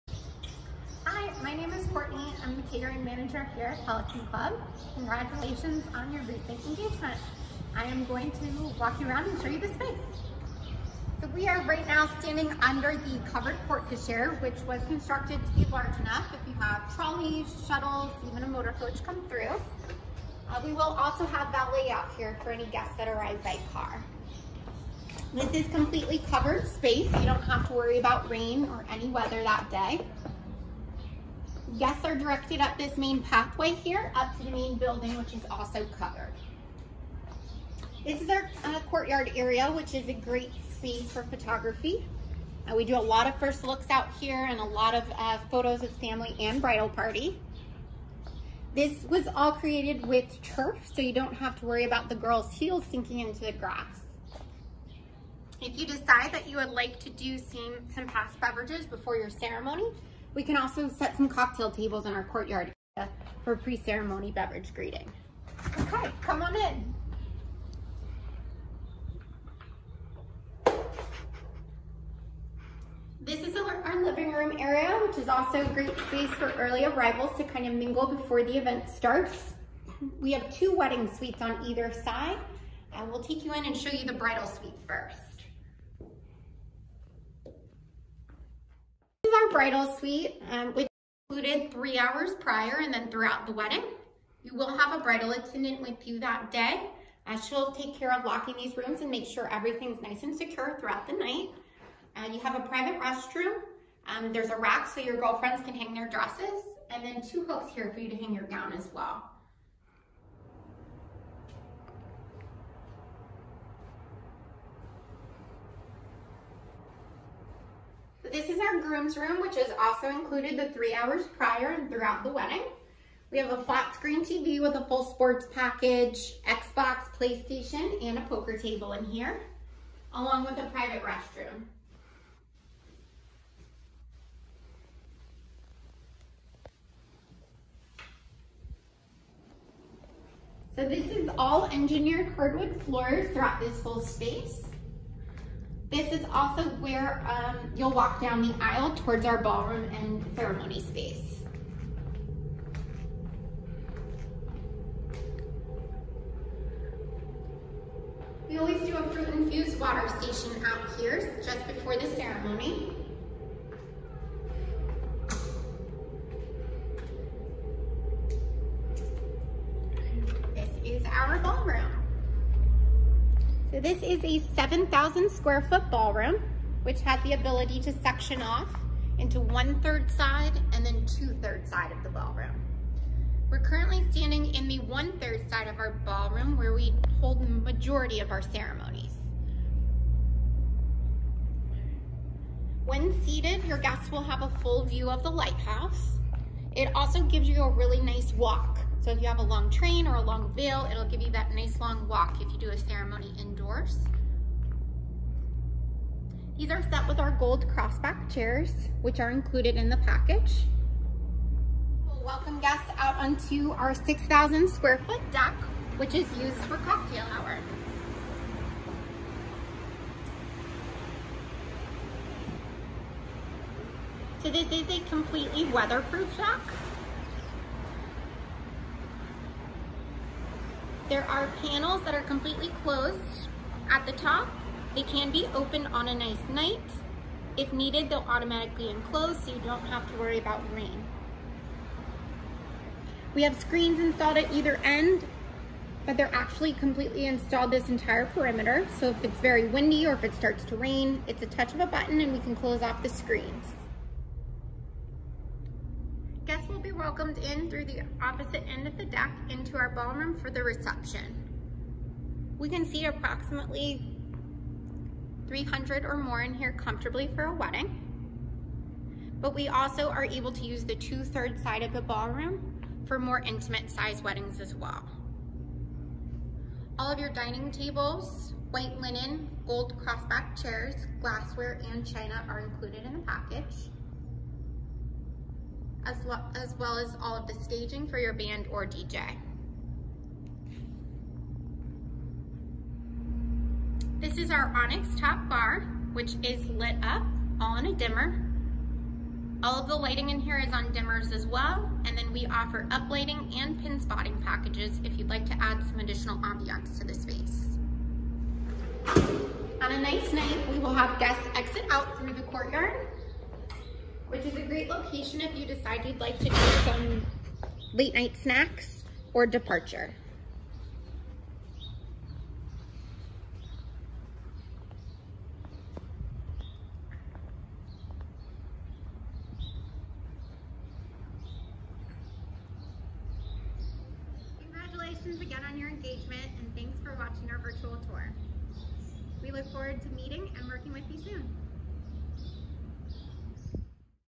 Pelican Club Virtual Tour
Courtney takes us through the Pelican Club.